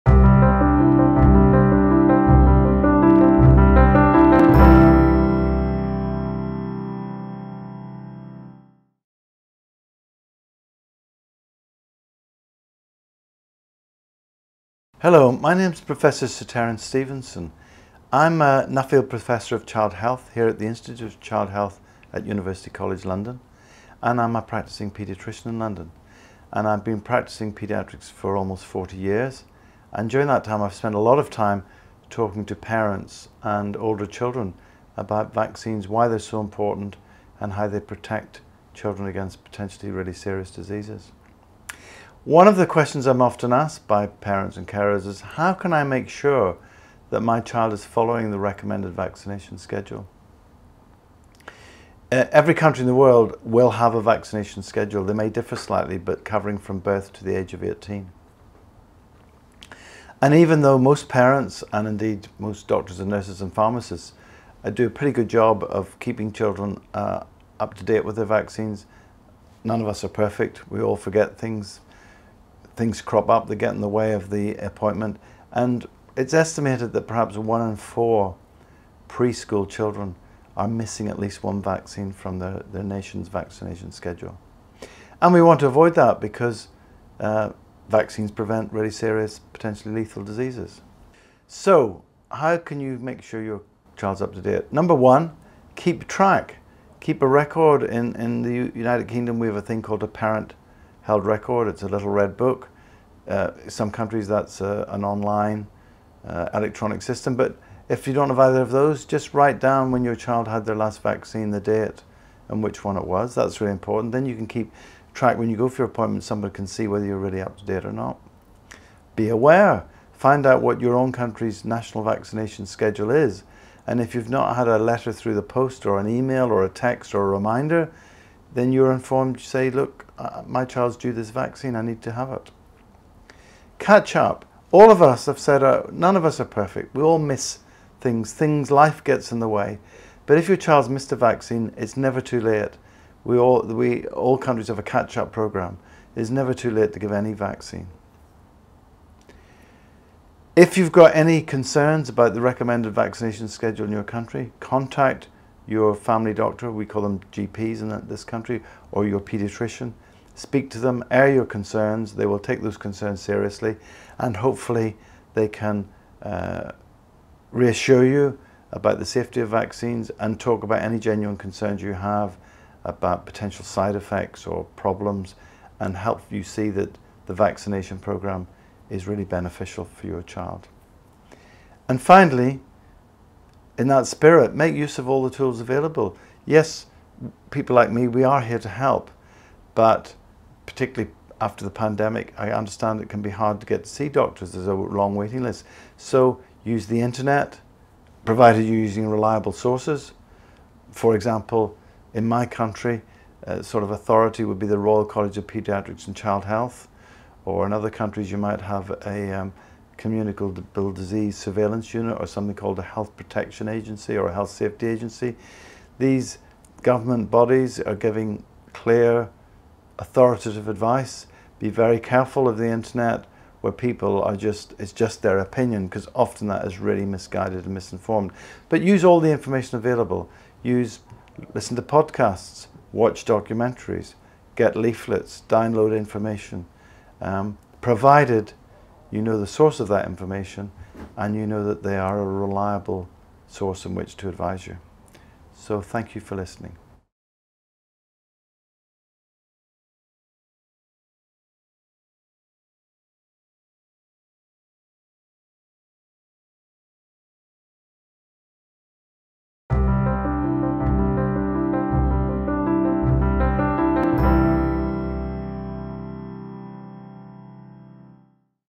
(PT) How Can I Make Sure My Child is Following the Recommended Vaccination Schedules?
Prof. Sir Terence Stephenson explains to parents how they can ensure their children are following the recommended vaccination schedules.

Prof. Sir Terence Stephenson is an Honorary Consultant & Nuffield Professor of Child Health at UCL GOS Institute of Child Health (UK)